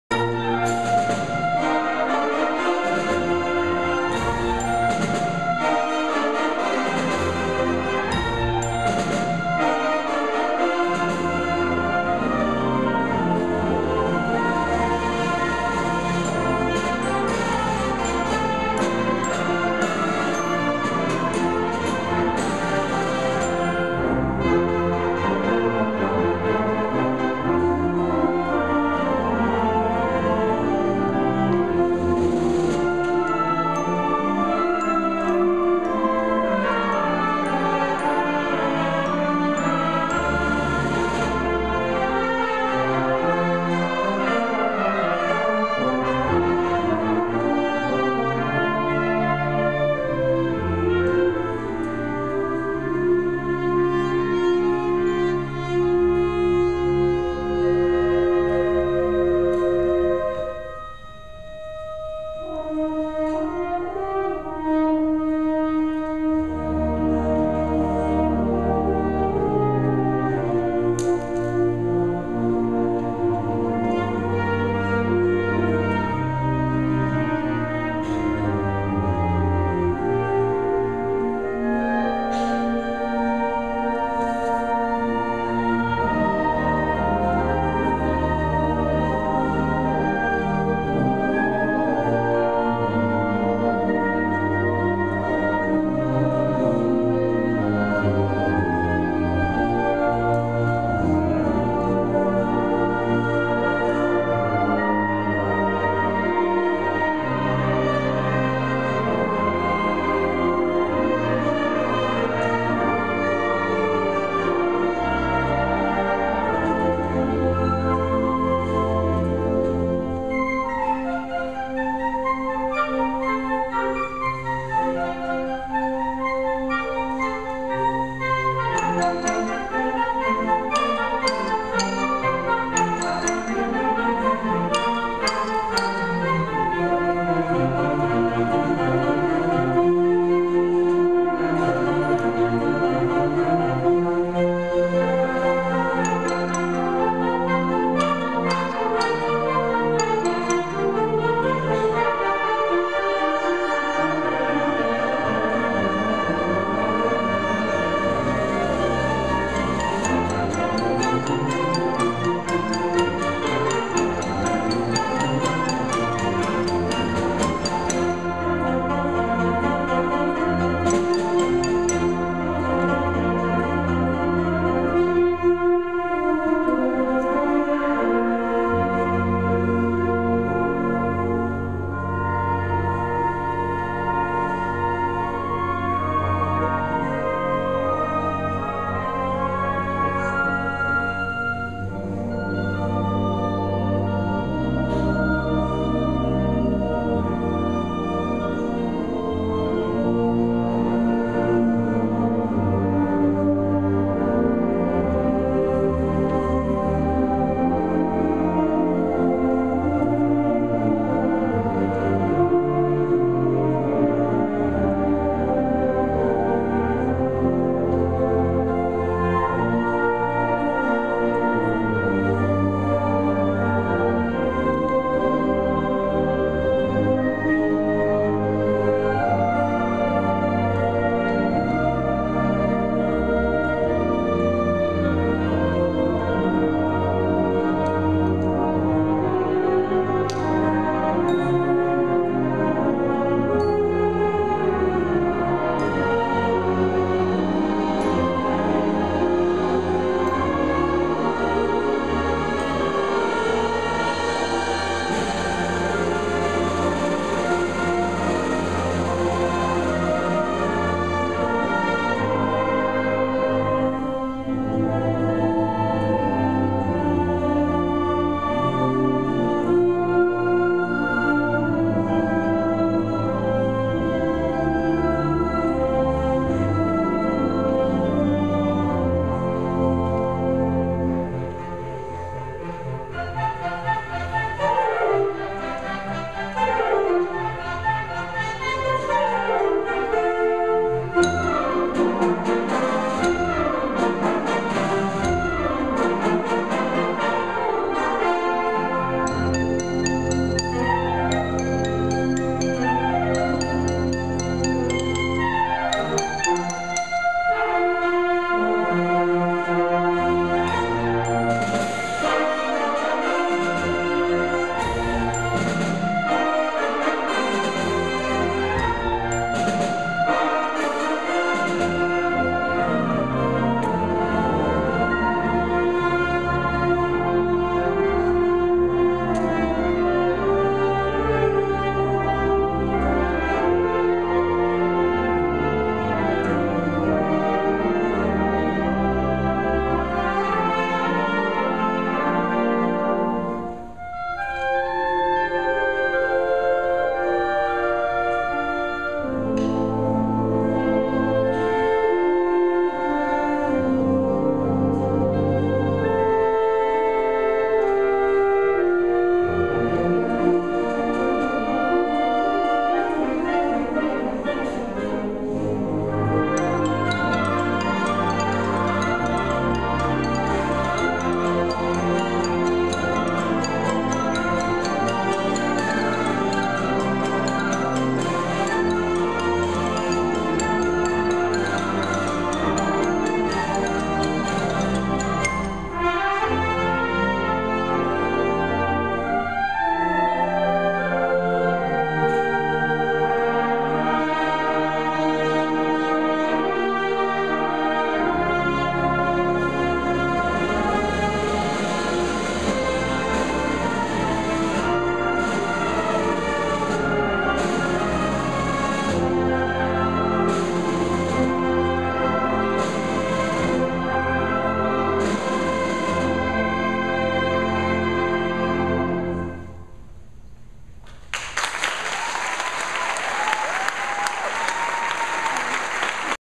FHS Band Spring Concert(combined FHS and FMS bands!)
A taste of what we could have next year...got a big class coming over from Fletcher Middle...and, it sounds like they're packin' HEAT! That's what happens when your band director is an International Man of Mystery!